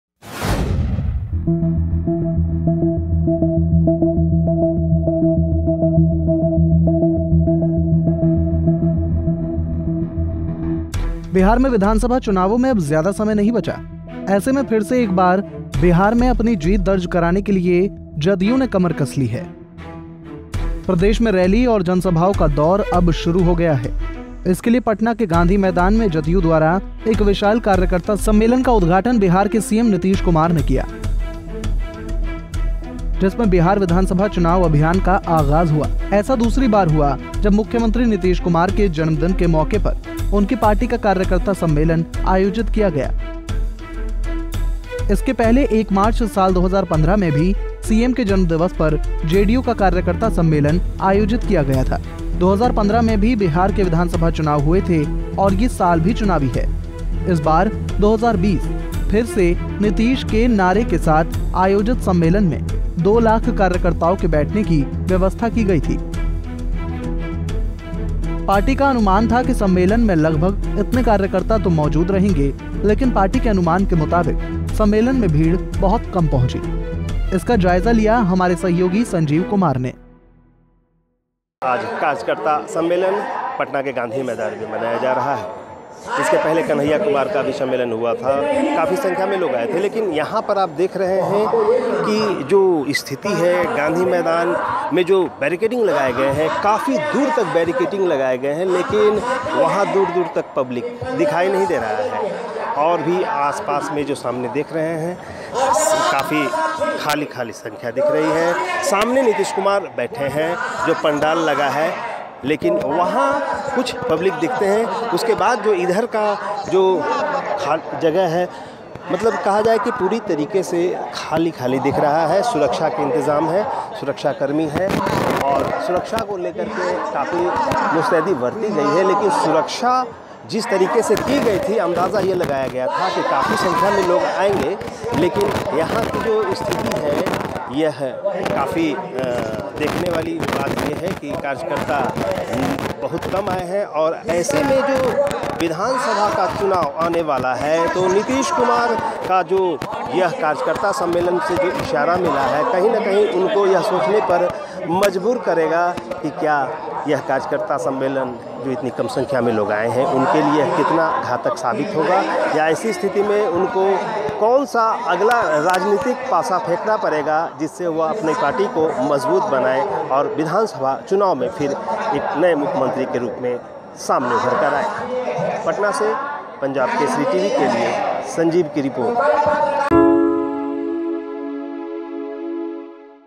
Patna: JDU के महासम्मेलन में उम्मीद से कम दिखी भीड़
पटना के गांधी मैदान में हुआ JDU का महासम्मेलन

सम्मेलन में 2 लाख लोगों के बैठने की व्यवस्था

सम्मेलन में उम्मीद से कम जुटी भीड़